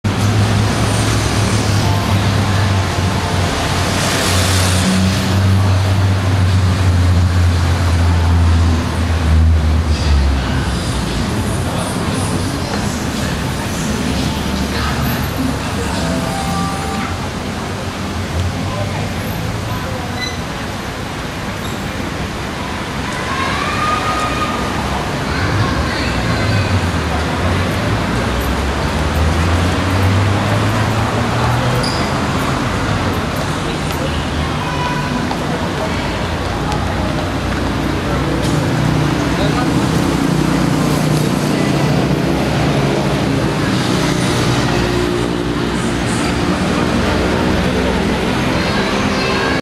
Bangkok Christian Hospital, Very Good Hospital in Bangkok, Thailand (Silom Road)
Where should you go when you are sick in Thailand,...which hospital? BCH is one of the best private Hospitals in the Country with many outreach centers, even in Chiang Mai. don't let the old façade fool you, inside is a very modern hospital.